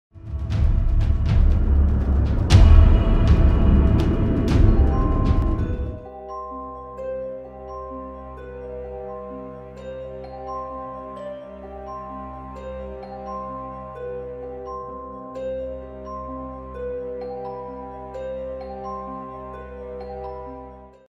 Trustinlies Gaming Intro + Outro
In lieu of the adventure map recording due to audio issues I'm sharing my new Intro and Outro with everyone.